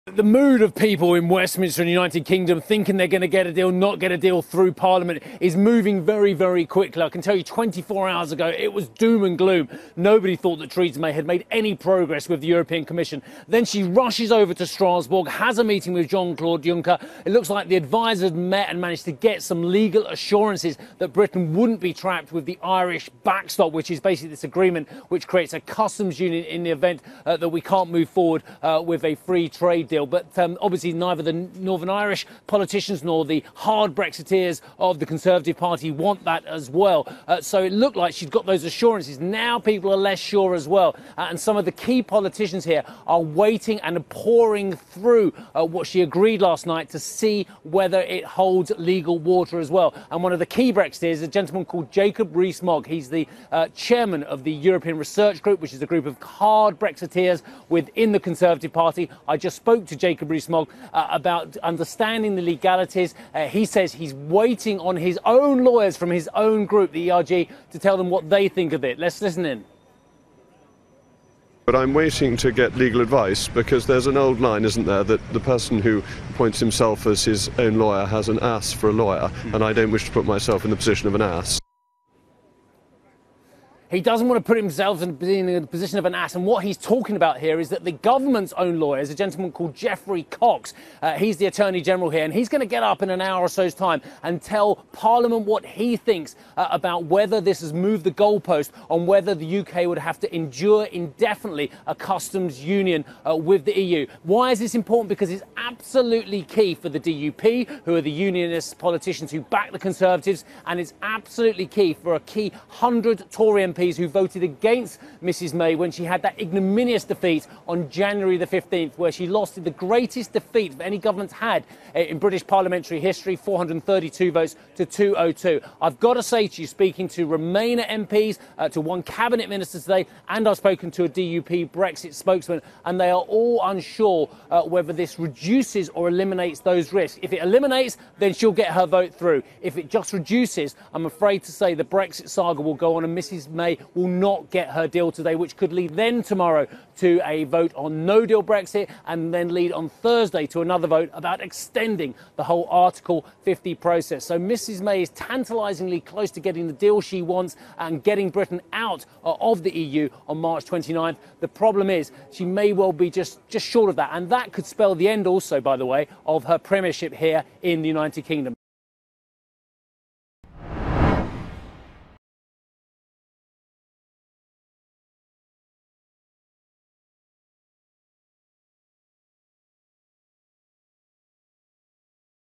UK lawmakers to vote on latest Brexit deal addressing Irish 'backstop'
British Prime Minister Theresa May announced legally binding assurances from the E.U. in a last-ditch effort to persuade parliament to back her exit deal. The Parliament is set to vote ahead of the March 29th deadline. CNBC's Steve Sedgwick reports.